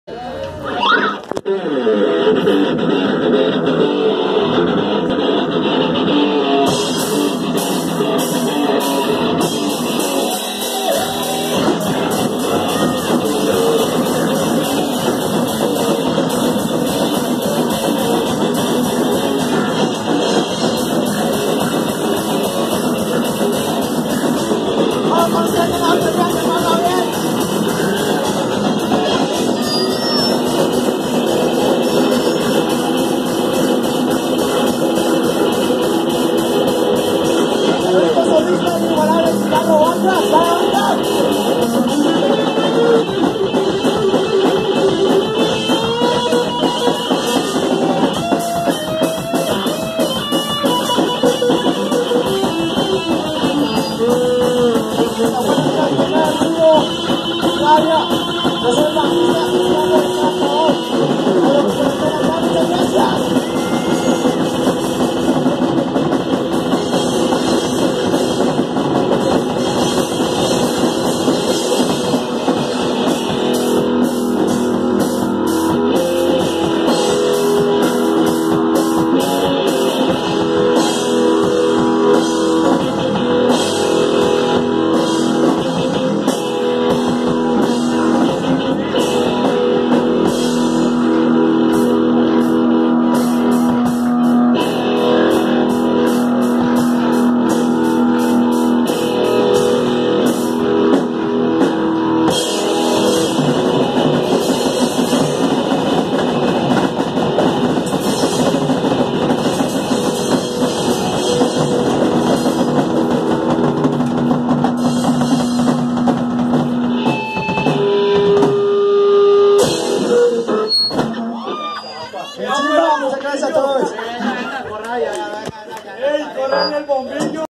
COLAPSO MENTAL - [en vivo, Underworld Fest vol.01] - parte 3
Presentación realizada el sábado 20 de febrero (2021) en la ciudad de Cali.
Toke: UNDERWORLD FEST VOL.01
Organizado  por Sala de Ensayo Black Dog

COLAPSO MENTAL
banda de metal punk de la ciudad de Cali (Colombia)